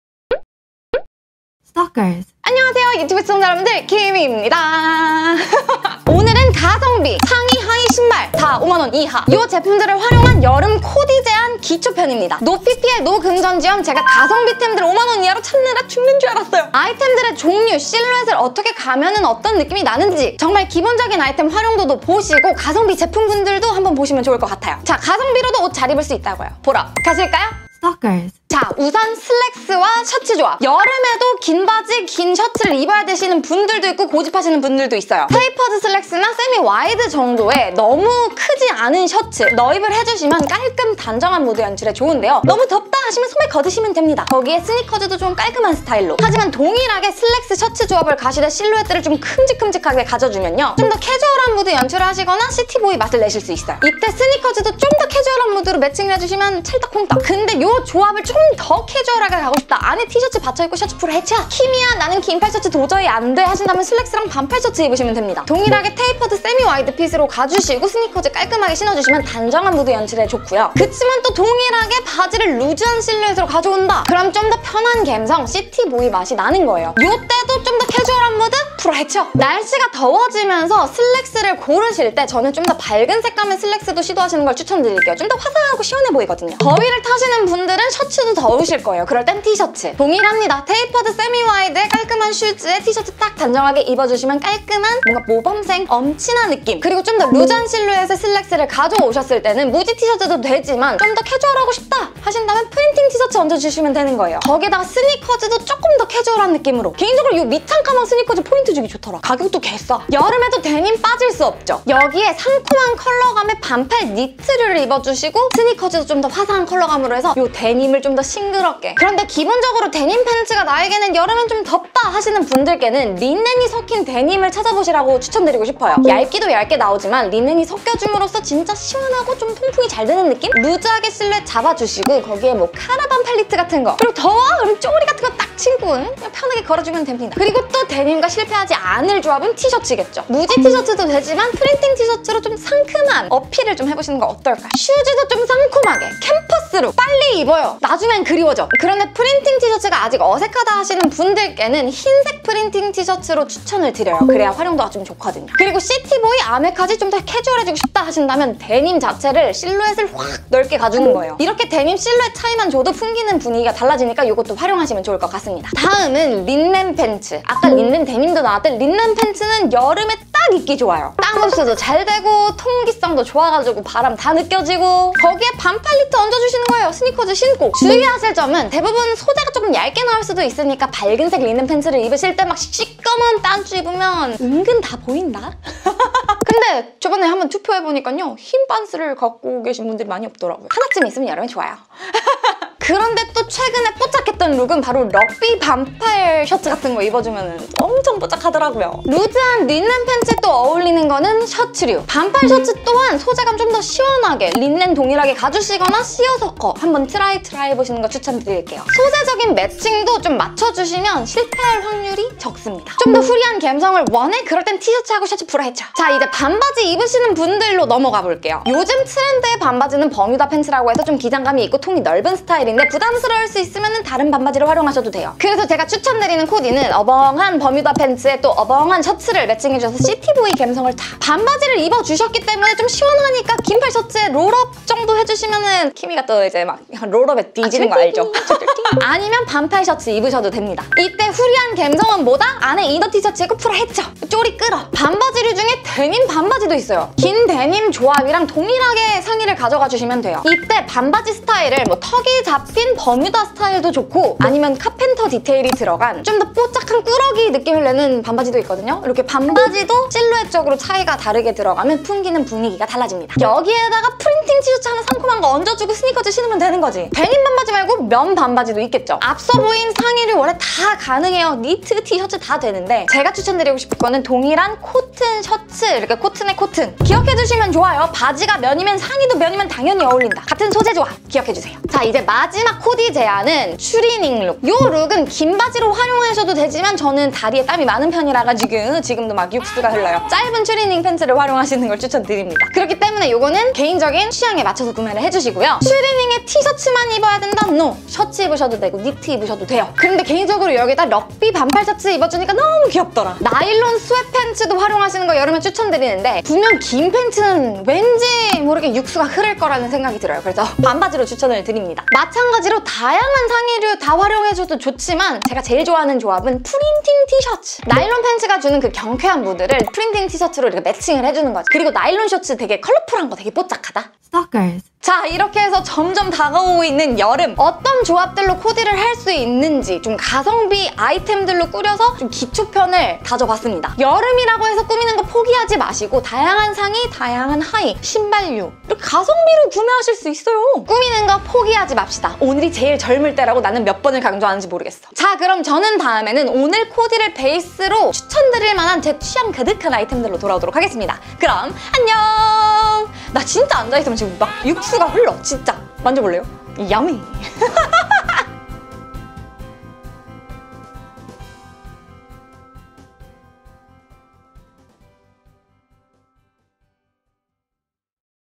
5만원 이하 여름 기본템을 활용한 20가지 코디법
***오늘 영상은 제가 한 제품, 한 제품 셀렉해서, 브랜드 요청을 드려서, 금전지원 없이, PPL 없이, 제품 지원만 받아서 만들어졌습니다**

*슈펜 슈탠다드 스니커즈가 공홈, 무신사에서  10% 할인 + 무료배송 혜택으로 29,900원→26,980원에 판매 하고 있습니다.

오늘은 5만원 이하의 제품들로 가성비 코디 영상을 준비했습니다!
더 저렴하게 구매 가능하기에 가성비템을 찾고 계셨던 분들께
조금이나마 도움이 되었으면 좋겠습니다ㅎㅎ

위의 링크 저희 카페 정리 글을 확인해주세요 :)

🌟MODEL INFO🌟 - 모델 스펙 183cm 70kg

0:00 오프닝
0:29 긴팔셔츠 + 슬랙스
0:49 오버핏셔츠 + 와이드슬랙스
1:04 반팔셔츠 + 슬랙스
1:28 반팔티 + 슬랙스
1:50 반팔니트 + 데님
2:17 프린팅반팔 + 데님
2:46 카라니트 + 린넨팬츠
3:20 반팔셔츠 + 린넨팬츠
3:26 여름 반바지 트렌드
3:48 셔츠 + 버뮤다팬츠
4:05 프린팅티 + 데님 반바지
4:28 셔츠 + 면 반바지
4:42 셔츠 + 츄리닝 반바지
5:04 프린팅티 + 나일론 반바지
5:24 엔딩